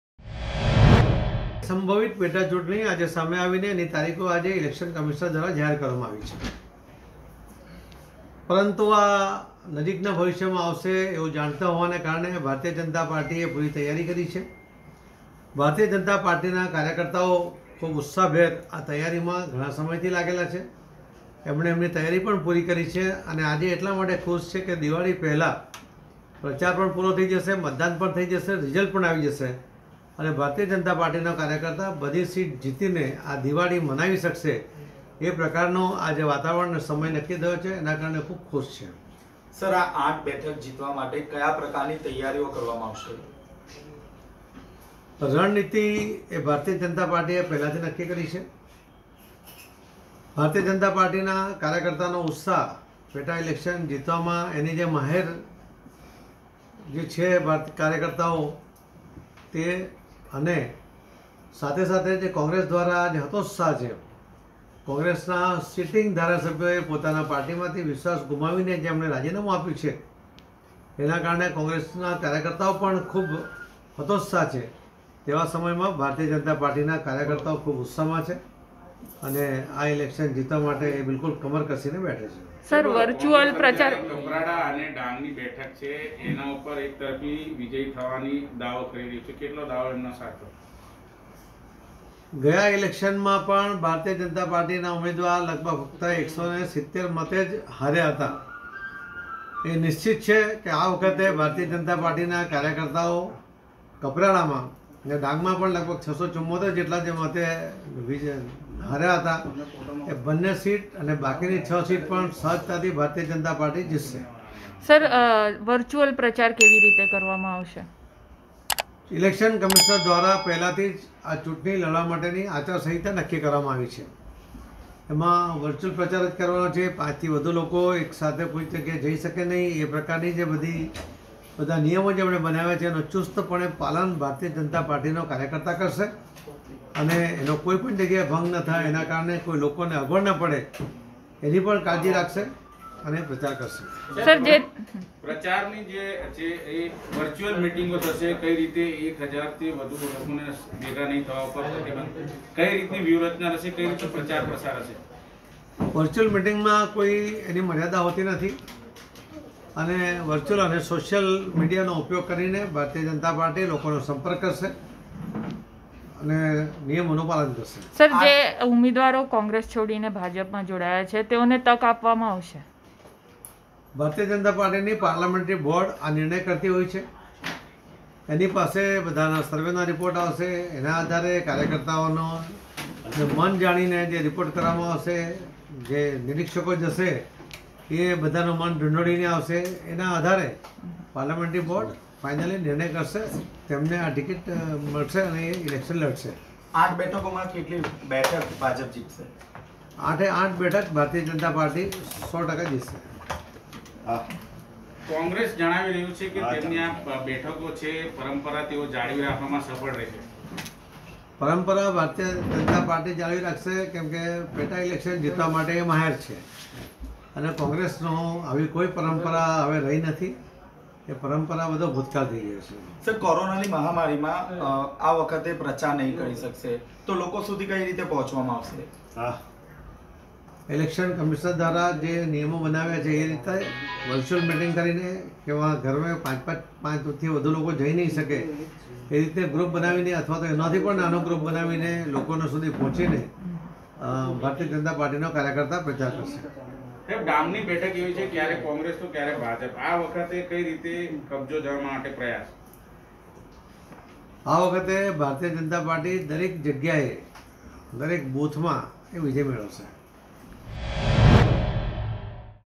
પાટિલની પરીક્ષાઃ આઠેય બેઠકો જીતવાનો દાવો, હાર્દિક સામનો કરવા સક્ષમ?
ભાજપના પ્રદેશ પ્રમુખ સી.આર. પાટીલે દાવો કર્યો છે કે ત્રીજી નવેમ્બરે રાજ્યમાં યોજાનારી આઠેય વિધાનસભાની બેઠક ભાજપ જીતશે અને આ વિજય પક્ષ માટે દીવાળીની ભેટ ધરીશું. તેમણે કહ્યું હતું કે કોંગ્રેસના જ વિધાનસભ્યો તેમનાથી નારાજ હોવાના લીધે તેમનો પક્ષ છોડી રહ્યા છે. તેની સામે ભાજપ આ બેઠક ખાલી પડી ત્યારથી તેને જીતવા માટે તૈયારી કરી રહ્યુ છે.